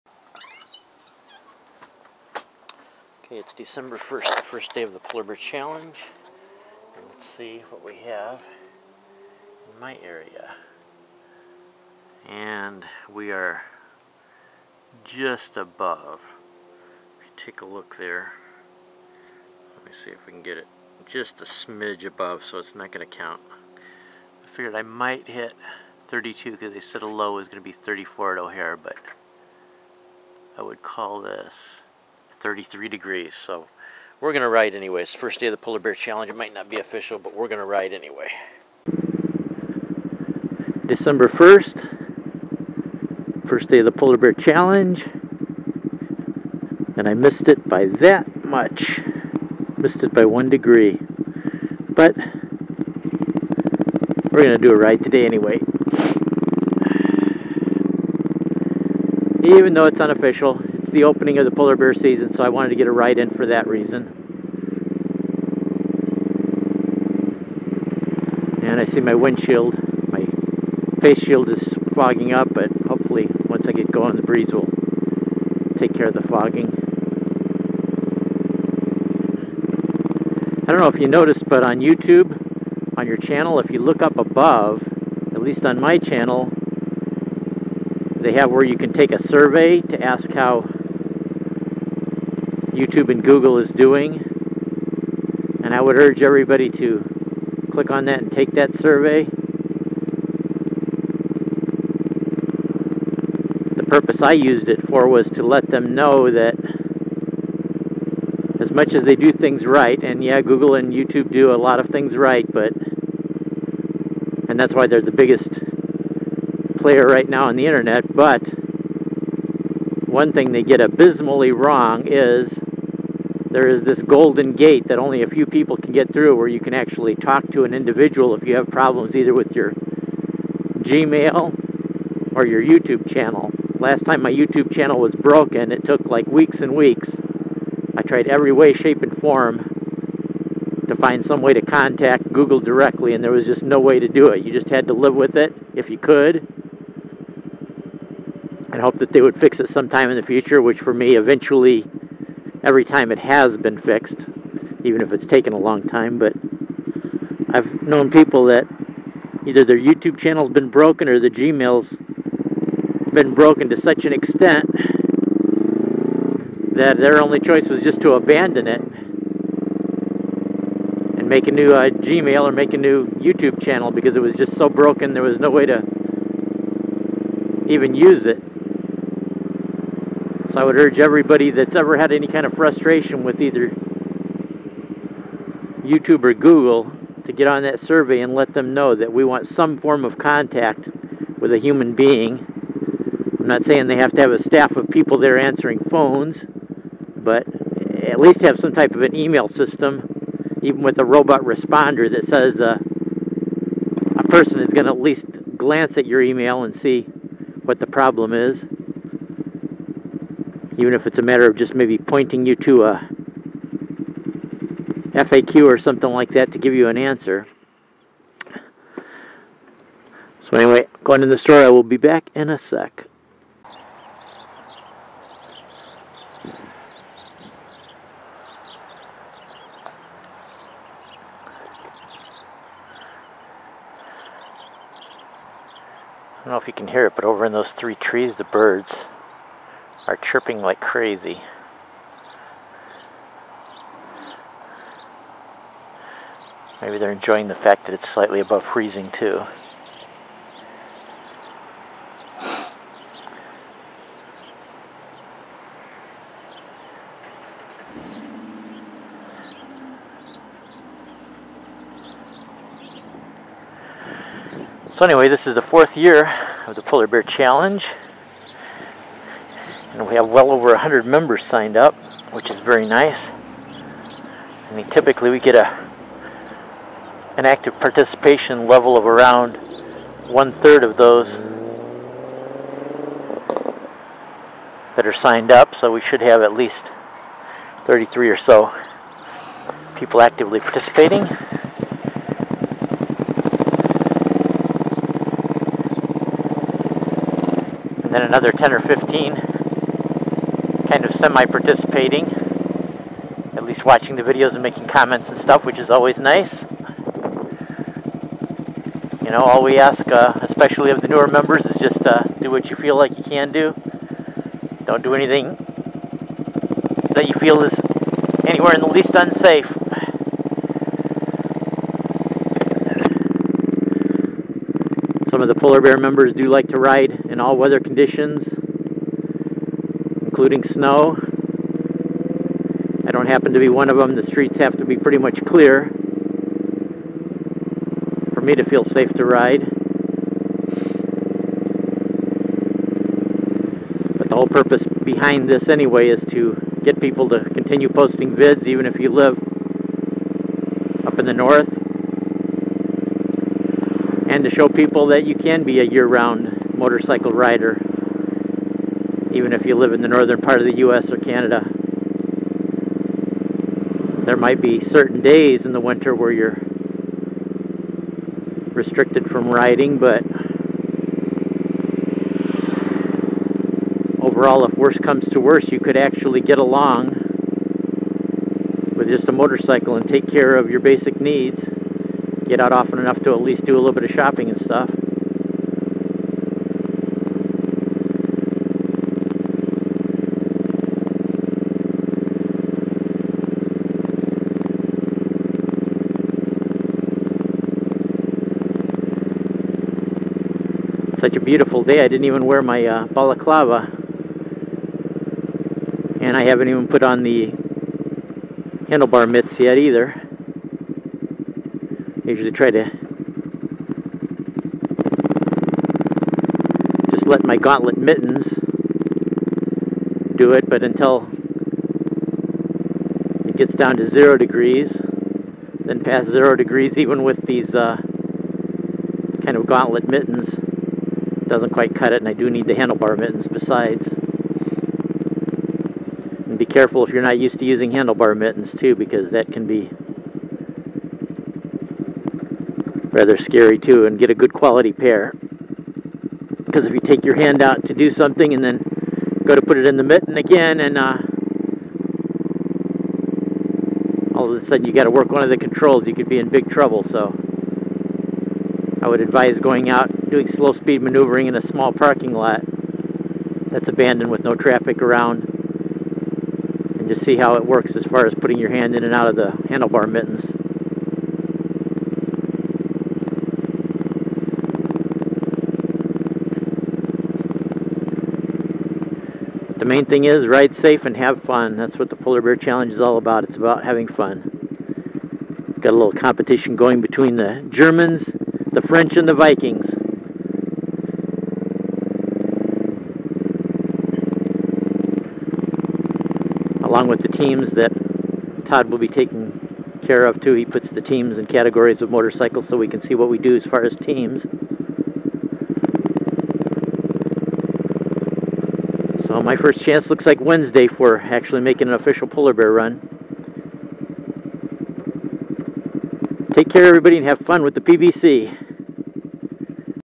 PBC Opening Day - missed by one degree :(
I make an opening day video for the Polar Bear Challenge.